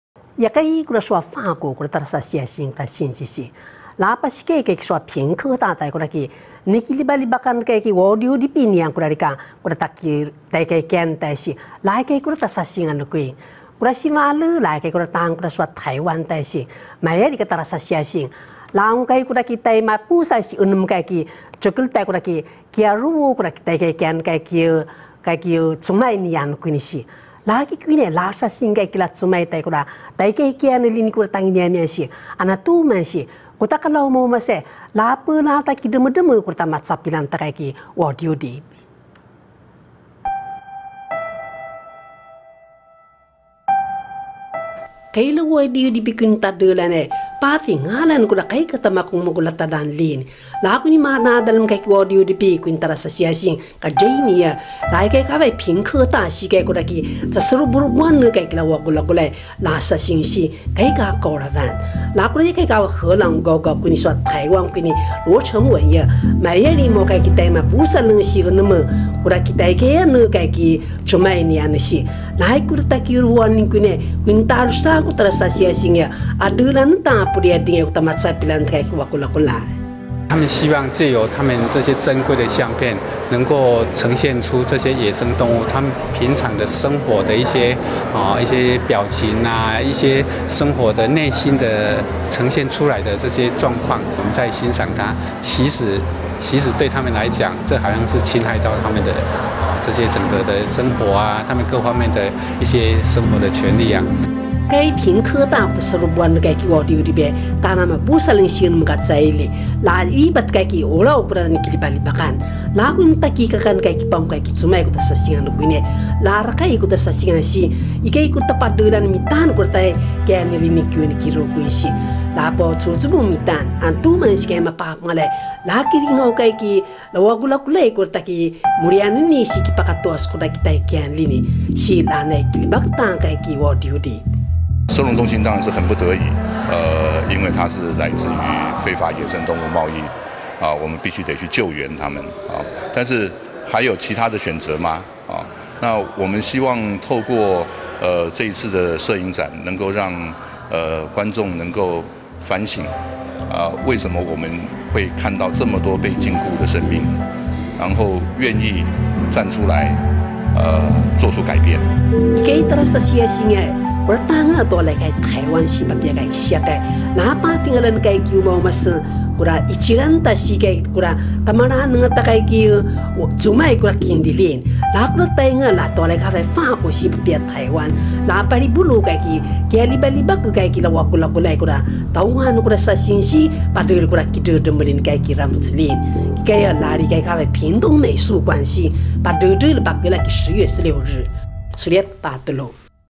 WILD-LIFE×白熊計畫 台法作品屏東展出 2018-10-13 Rukai IPCF-TITV 原文會原視族語新聞
tarasasing ki Taiwang si Hwaku padredrele ku cumay

每個動物的凝視，透露出被禁錮的無奈，喜愛動物的法國攝影師Jimmy，在旅台期間為「屏科大保育類野生動物收容中心」的動物，拍攝了一張張緊扣人心的照片。而旅居荷蘭攻讀藝術碩士的台灣攝影師羅晟文，則是走訪26個圈養北極熊的動物園，拍下北極熊被禁錮的情景。兩位攝影師的作品，讓人們重新思考，人與動物的關係。

屏科大野生動物收容中心成立26年，收容過上萬隻的保育類動物，「WILD-LIFE×白熊計畫」聯合攝影展，用鏡頭將動物遭禁錮的影像，帶到大眾面前，希望觀看的民眾，體會野生動物，被迫離開棲地的恐懼與悲哀，進而為野生動物保育盡一份心力。

兩位攝影師，一位從台灣走向世界，凸顯因人類的自私，剝奪了北極熊的權利；一位從法國走進台灣，從協助野生動物的想法切入，一幅幅撼動人心的攝影作品，將於屏東美術館展出至10月16日。